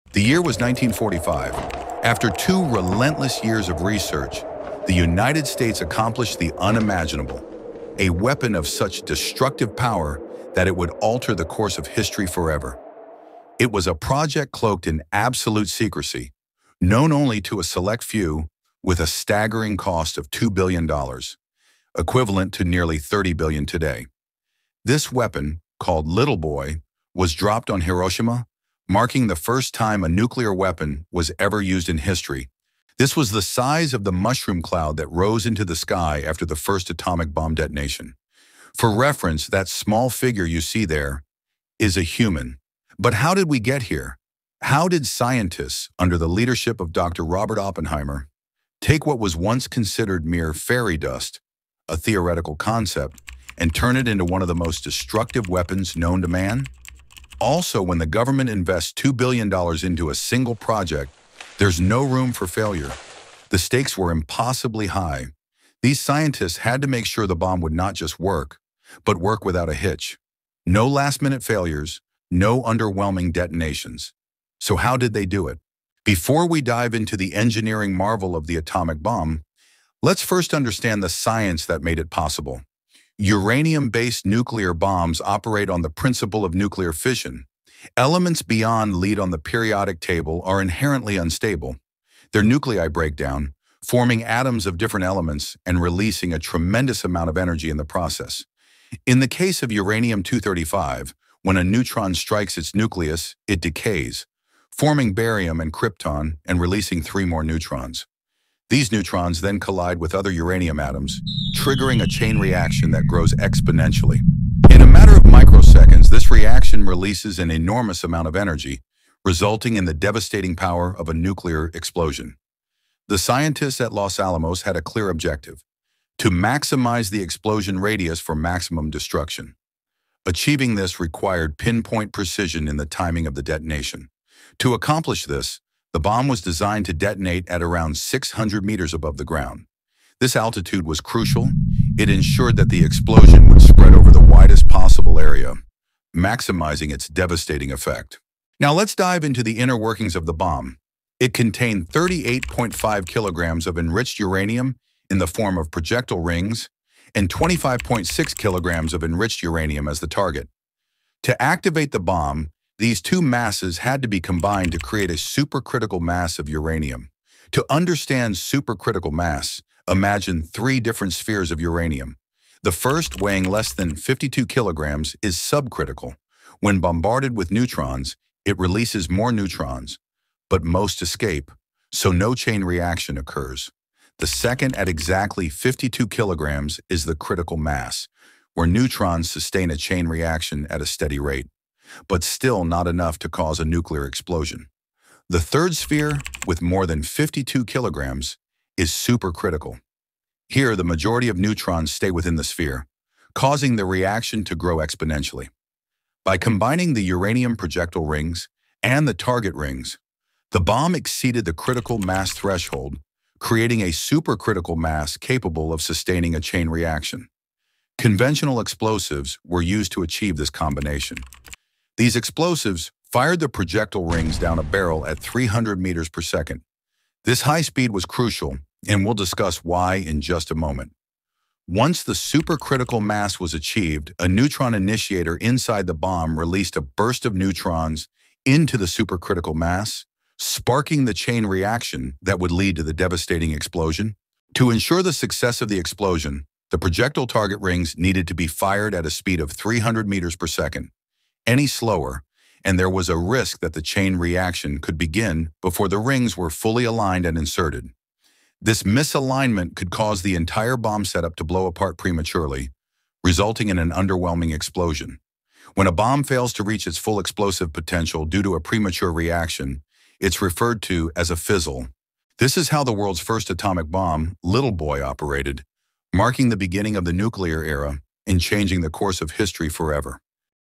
How Does A Nuclear Bomb Work? (3D animated)
In this video, we explain how a nuclear bomb works in the simplest way possible. You'll learn what makes these bombs so powerful and how they release energy.

This is a basic, beginner-friendly explanation of nuclear fission and the science behind the explosion.